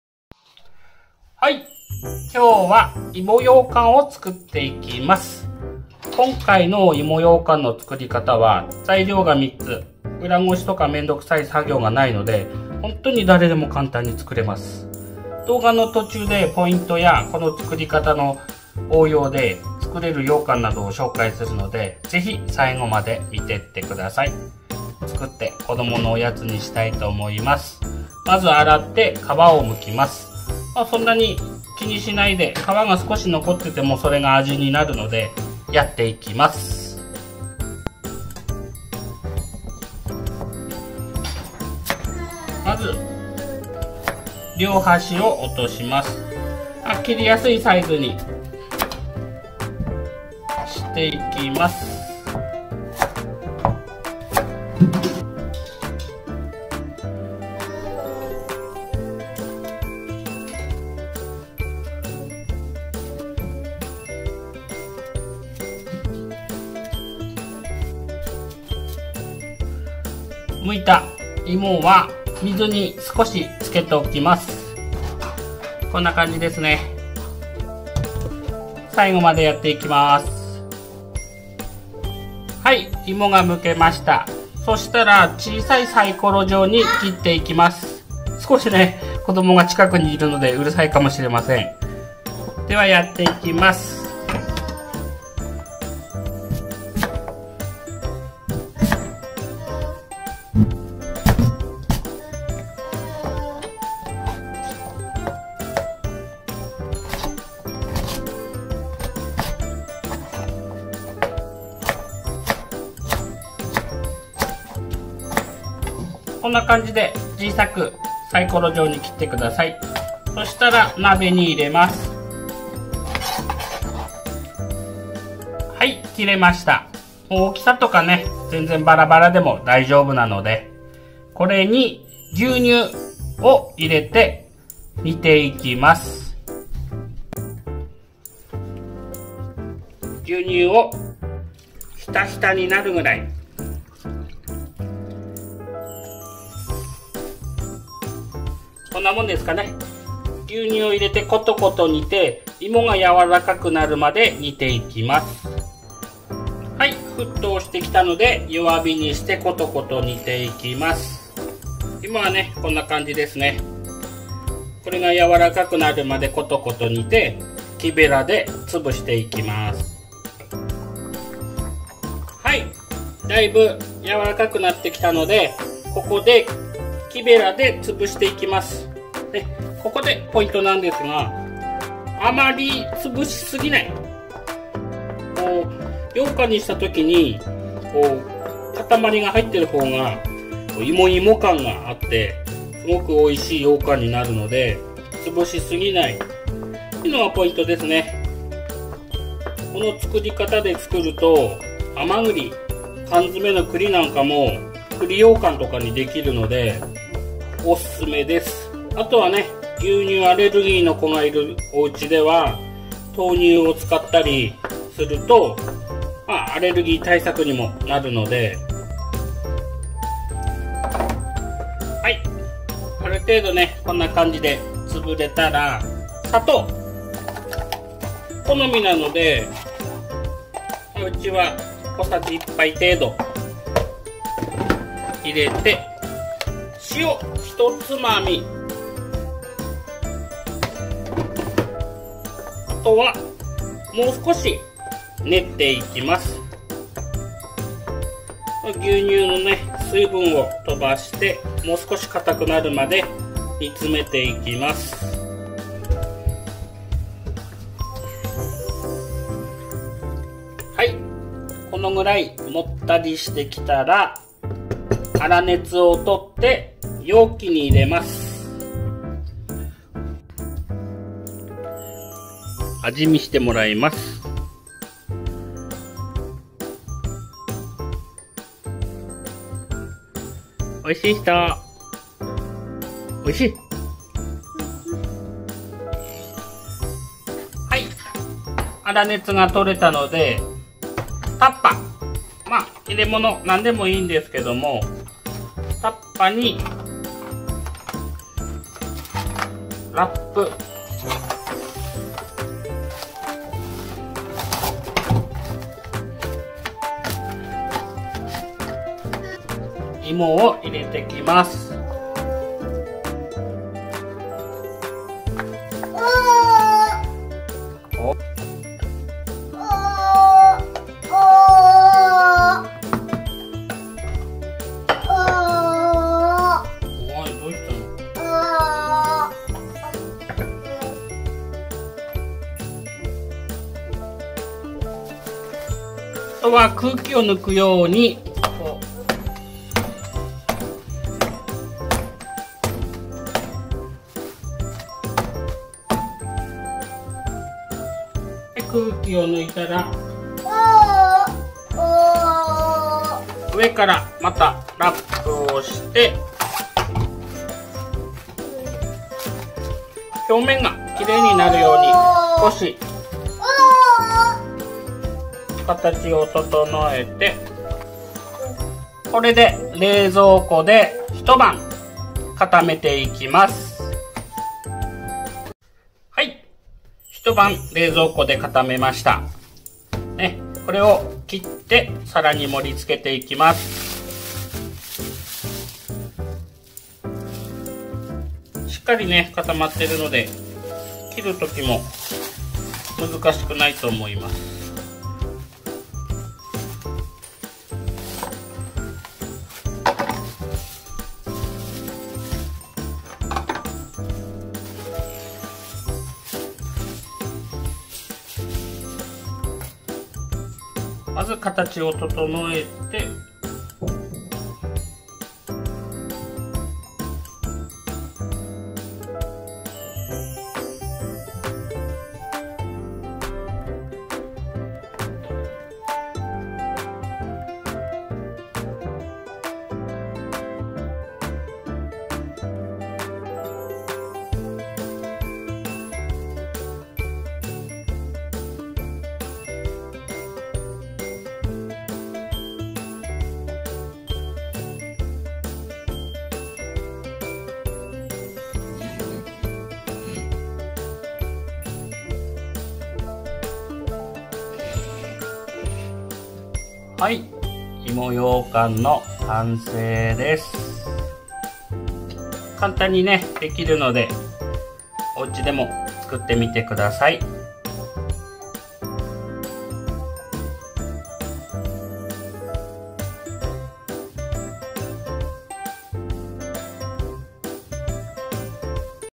材料3つ！めんどくさい作業無し！簡単【芋ようかん】
今回は材料が3つだけで作る、超簡単な芋ようかんを作っていきます！芋ようかんだけではなく【栗ようかん】もこの作り方で作れます😋牛乳アレルギーの子がいるお家は、【豆乳】でも作れますよ☝️
子供が近くで遊んでいるのでアァ～アァ～って声がはいりますが、暖かい気持ちで見てください😅

材料
サツマイモ
牛乳
砂糖
塩(１ツマミなのでノーカウント)で(笑)